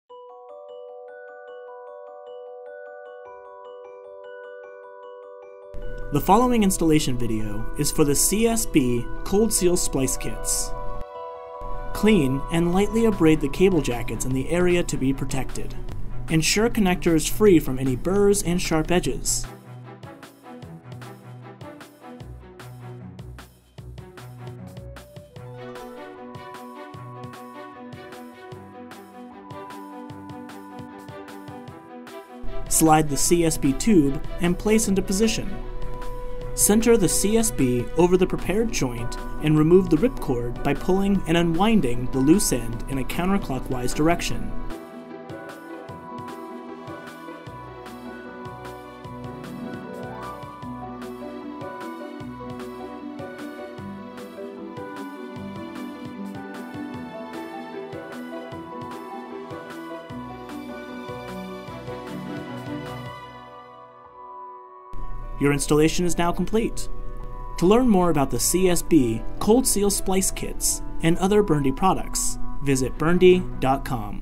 CSB Cold Seal Splice Kit Installation Video
In this short installation video, we'll guide you through installing the CSB Cold Seal Splice Kit from BURNDY.